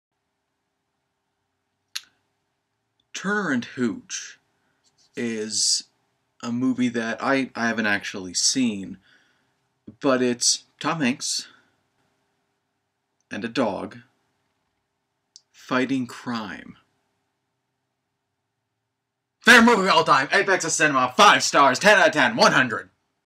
Turner and Hooch Review*
*not really
This was literally my reaction to learning of this glorious film's existence. I'll have to watch it some time.

Tags (ignore): turner and hooch, tom hanks, review, vulcan's forge films, sean capdeville